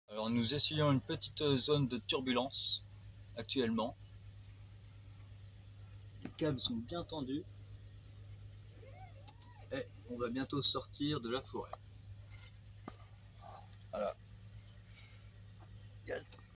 fourgon et tracteur 05
on est content, ça se passe bien, les sangles et les mousquetons tiennent bon...